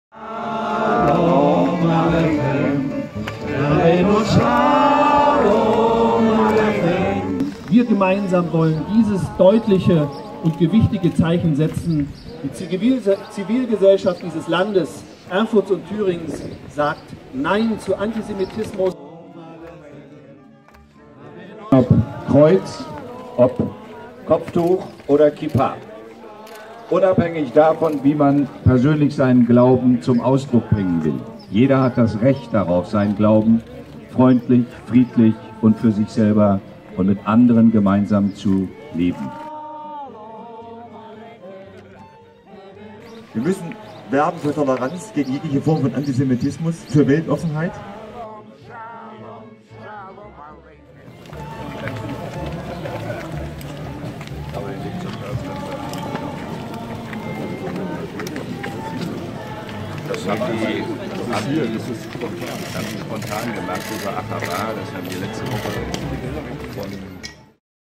Thüringen trägt Kippa, Erfurt 25.4.2018
Unter dem Motto „Thüringen trägt Kippa“ bekennen sich zahlreiche Menschen in Erfurts Innenstadt zu Religionsfreiheit und Solidarität mit diffamierten Juden in Deutschland und anderen Ländern. Wir sind dabei!